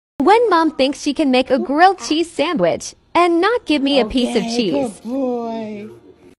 Does my HUGE puppy like grilled cheese
The Big Good boy puppy Dog Bowser has some serious love for CHEESE it turns out, especially grilled has another day of adventures of being a giant who always loves a treat and attention. It's not easy being giant but Bowser does it well!